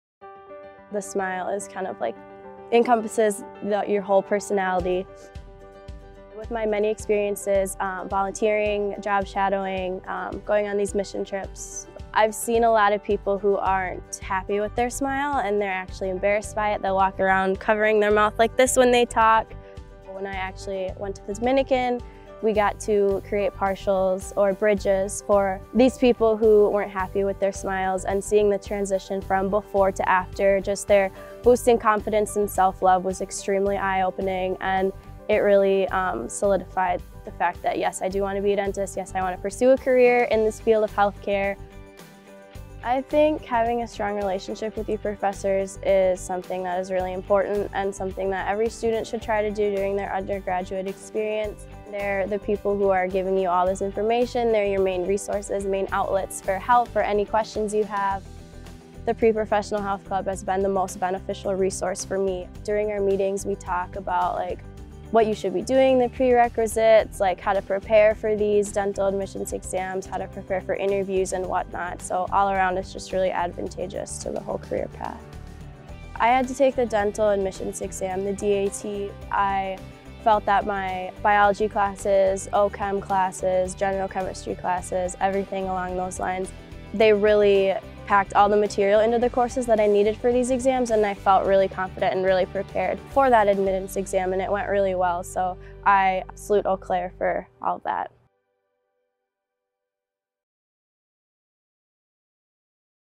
Pre-dentistry
Oral health is a crucial component to one's overall health. UW-Eau Claire students who declare a pre-dentistry track will learn alongside exceptional faculty and career advisors, who will guide one through dental school requirements and ensure one gains the knowledge and experiences necessary for acceptance.